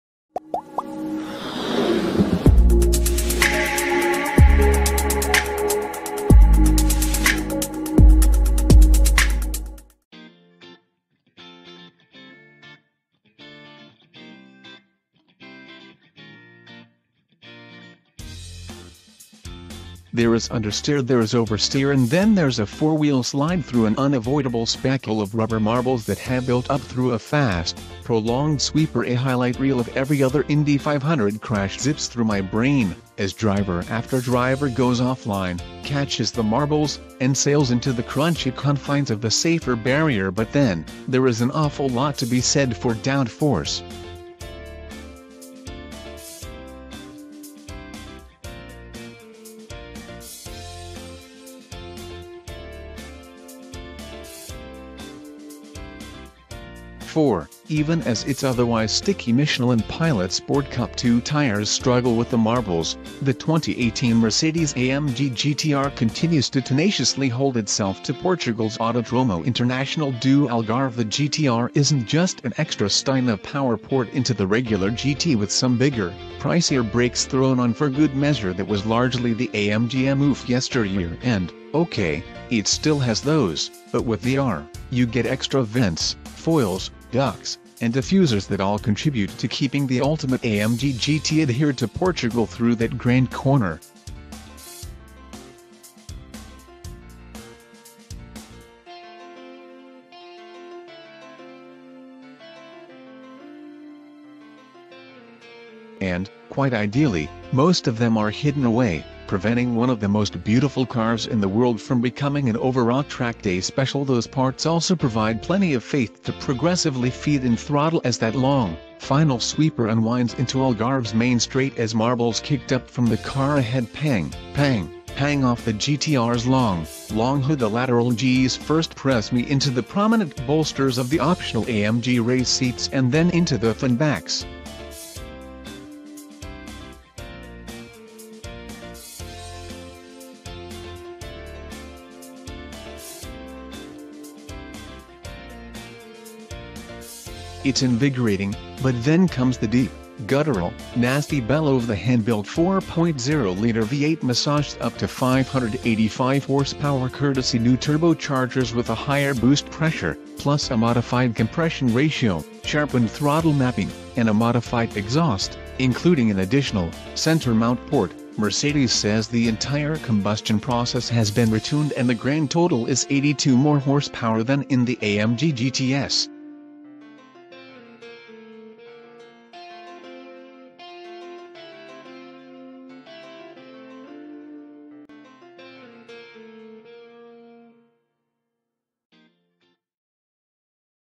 2018 Mercedes-AMG GT R - Glance and Aerodynamics Review (Beast of the Green Hell) REVIEW
There is understeer. There is oversteer. And then there's a four-wheel slide through an unavoidable spackle of rubber marbles that have built up through a fast, prolonged sweeper. A highlight reel of every other Indy 500 crash zips through my brain, as driver after driver goes off line, catches the marbles, and sails into the crunchy confines of the Safer Barrier.  But then, there is an awful lot to be said for downforce. For, even as its otherwise sticky Michelin Pilot Sport Cup 2 tires struggle with the marbles, the 2018 Mercedes-AMG GT R continues to tenaciously hold itself to Portugal's Autodromo Internacional do Algarve. The GT R isn't just an extra stein of power poured into the regular GT with some bigger, pricier brakes thrown on for good measure. That was largely the AMG M.O. of yesteryear. And, okay, it still has those, but with the R, you get extra vents, foils, ducts, and diffusers that all contribute to keeping the ultimate AMG GT adhered to Portugal through that grand corner. And, quite ideally, most of them are hidden away, preventing one of the most beautiful cars in the world from becoming an overwrought track-day special.

Those parts also provide plenty of faith to progressively feed in throttle as that long, final sweeper unwinds into Algarve's main straight as marbles kicked up from the car ahead pang, pang, pang off the GT R's long, long hood. The lateral G's first press me into the prominent bolsters of the optional AMG race seats and then into their thin backs. It's invigorating, but then comes the deep, guttural, nasty bellow of the hand-built 4.0-liter V8 massaged up to 585 horsepower courtesy new turbochargers with a higher boost pressure, plus a modified compression ratio, sharpened throttle mapping, and a modified exhaust (including an additional, center-mount port). Mercedes says the entire combustion process has been retuned and the grand total is 82 more horsepower than in the AMG GT S.    Yet, the resulting acceleration advantage isn't just a matter of extra power (the 0–60 run is estimated at 3.5 seconds versus 3.7 of the GT S). Few areas of the GT R have avoided the weightsaver's scalpel. This includes the wider front fenders, roof, torque tube, drive shaft, and multiple structural braces made of carbon fiber, plus forged wheels and composite materials taking the place of several metal parts. Mercedes had most of these components on hand at Algarve to let us hoist them ourselves, and the weight difference is obvious – lifting the nine-pound driveshaft makes you look like The Rock – and it's easy to see how all of it together shaves off about 200 pounds versus the GT S's curb weight.

 A further 37 pounds can be cut by opting for the carbon-ceramic disc brakes, which provide fade-free, mammoth stopping power as the main straight dips downhill before leading into a sharp right turn. They are the definition of confidence-inspiring despite the very-Mercedes low-effort pedal that makes for perfectly pleasant real-world driving.  Really, though, it's the gentlemen in lab coats manning the wind machine that have made the biggest difference. It all begins up front. Those 15 chrome slats that boldly differentiate the R from its GT siblings and evoke the original 1950s SL race cars actually disguise the fact the grille has been tilted forward for improved aerodynamics. Beneath it is the "active air management system," or a wide box filled with vertical slats. They automatically open when additional cooling is needed, but mostly stay closed to reduce drag and direct air down under the car to the next bit of engineering wizardry: the exclusive new active aerodynamics profile.

When the car is in Race mode at speeds higher than 50 mph, a carbon-fiber blade lowers by 1.6 inches and significantly changes airflow. This reduces front-axle lift by essentially sucking the car to the ground and leads to improved high-speed steering and stability. There wasn't a GT S on hand to compare to this improved bit, but there certainly is never a reduction in confidence from the GT R's steering, which lets you sharply and fluidly turn into corners. But more on that shortly.  As the air moves under the GT R, it's channeled to the special rear diffuser or up to the air outlet between the taillights (shared with the one-rung-lower GT C).